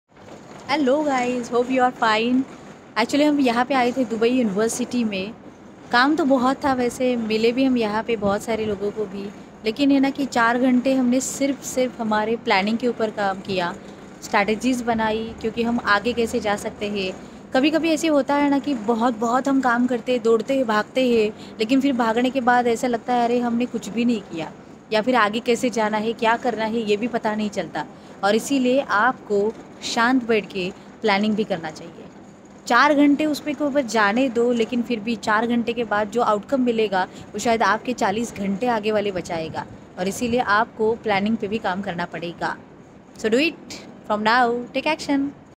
Export Planning I Export OpportunitiesI KDSushma
Why planning is Important in the Export Business?

Learn, Implement and Take Action

ON A MISSION TO CREATE 10000+ EXPORTERS BY 2025
Take Action and start your Export Journey.